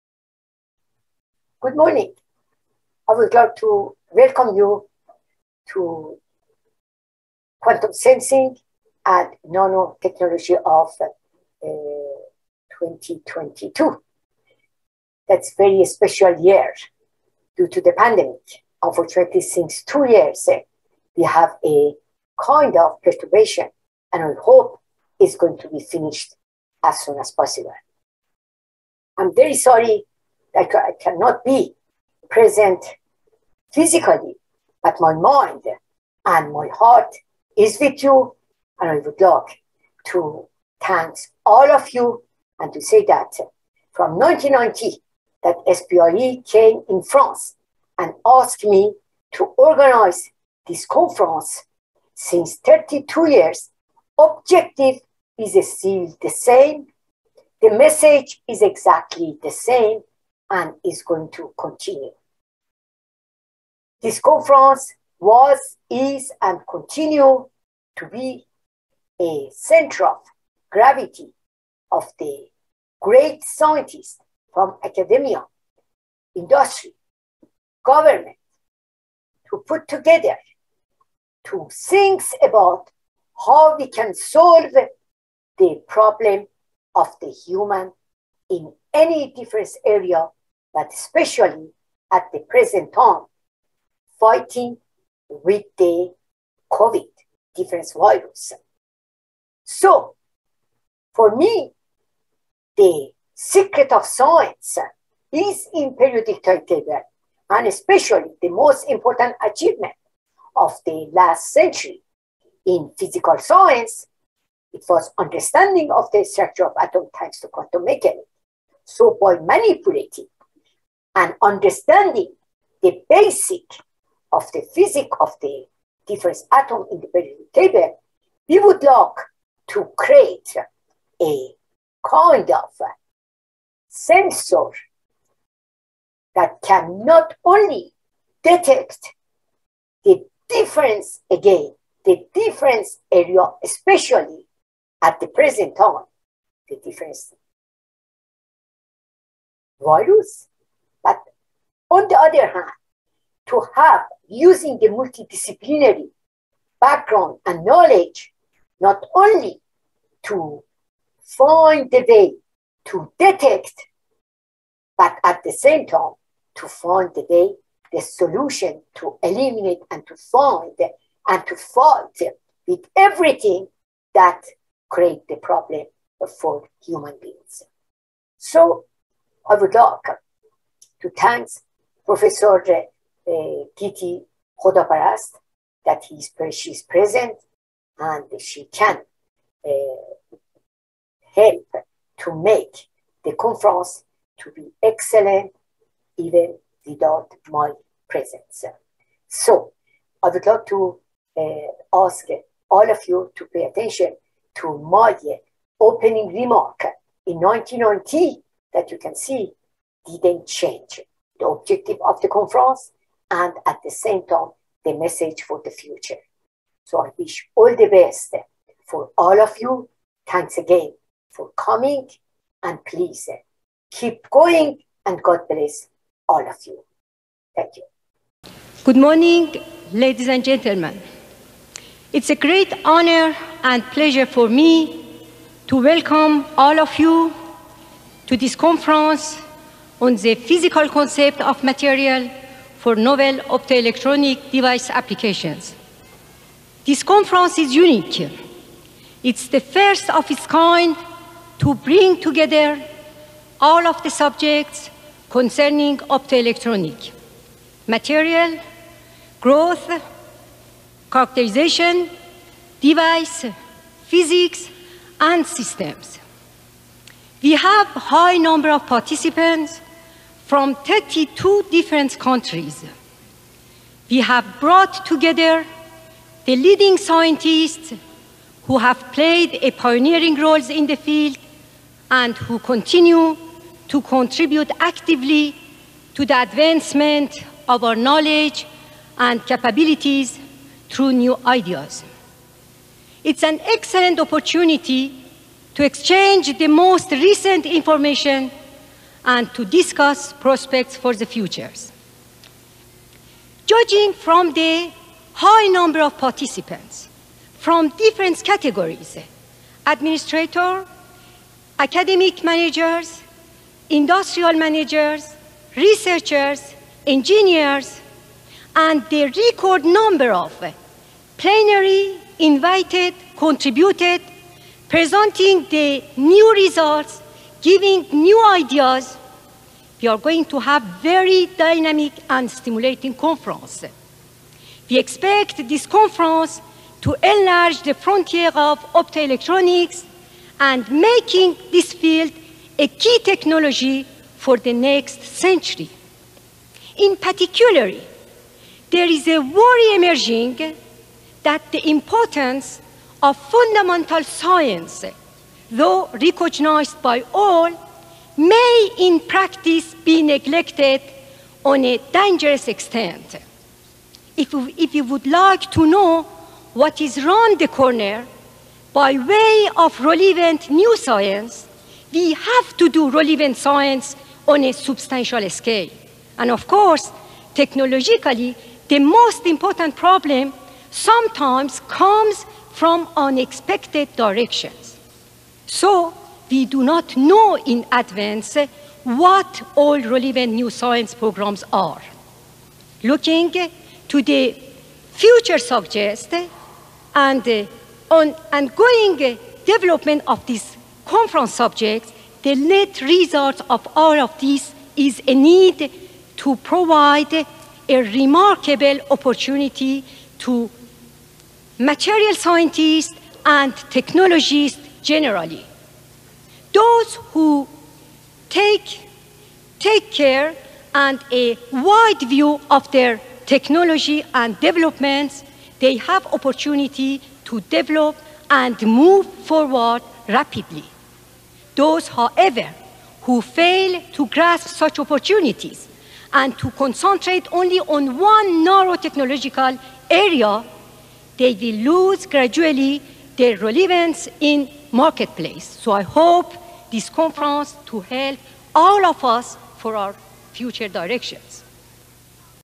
2022 SPIE Photonics West -- Introductory Remarks
Introductory remarks to Professor's Razeghi's legendary Quantum Sensing Conference at SPIE Photonics West, now in its 30th year in 2022.